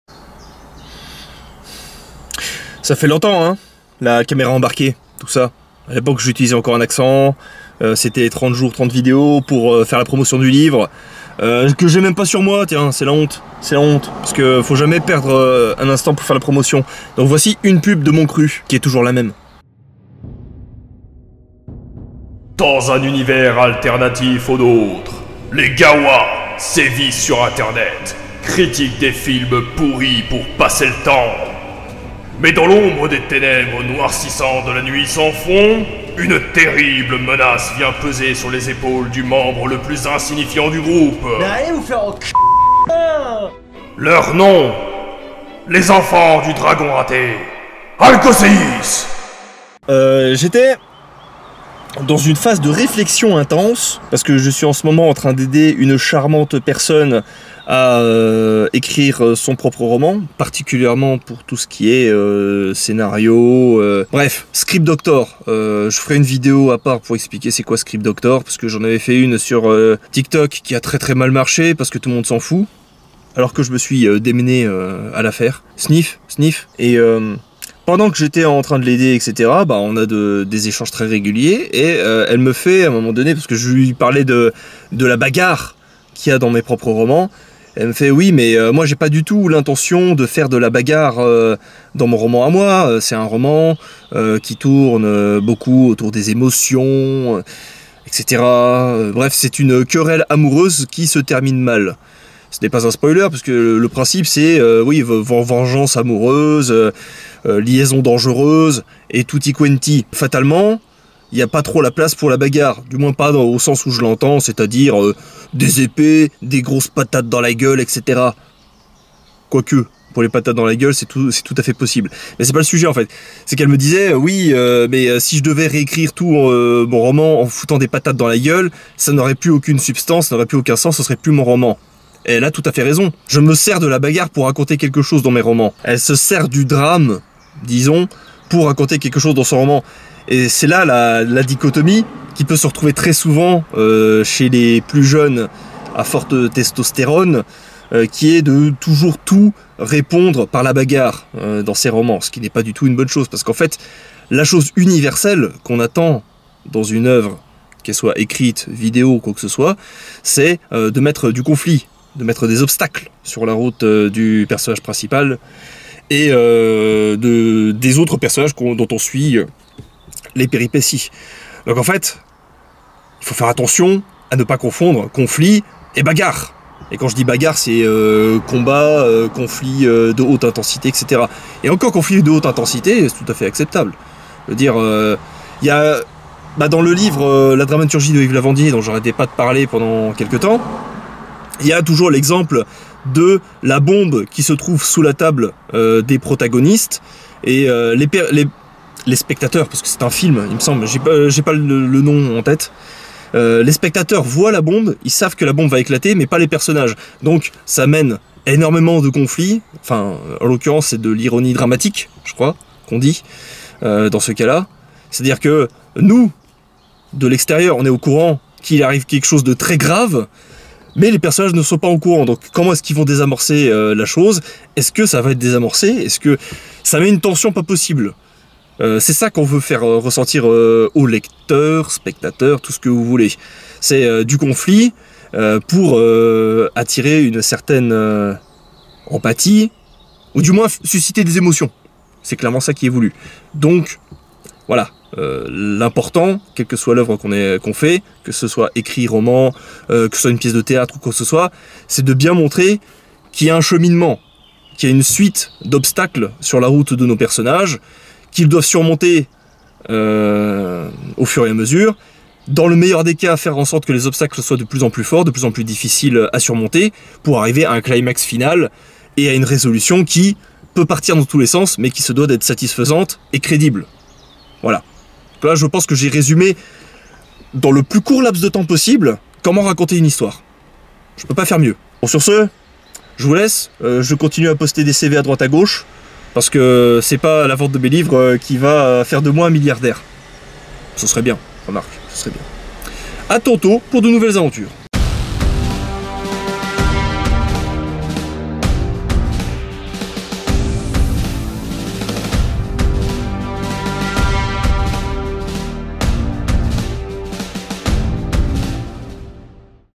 Il faut forcément de la bagarre !
Salutations !
Entre cinq poses de CV, je m'arrête en bordure de forêt, en pleine inspiration...

Travaillant actuellement à aider une jeune auteure - la première d'une longue liste pour un débutant script-doctor - à publier son premier roman, la question ironique de la bagarre s'est posée : "Non, y'a pas de bagarre dans mon roman et j'en veux pas".

En réalité, on s'est mal compris. Il était question de mettre l'accent sur le "conflit" et non la pure baston. La différence ? Je l'explique dans cette vidéo.

Mon roman "Les Gawaa tome 1, l'héritage du dragon raté" disponible en format relié et Ebook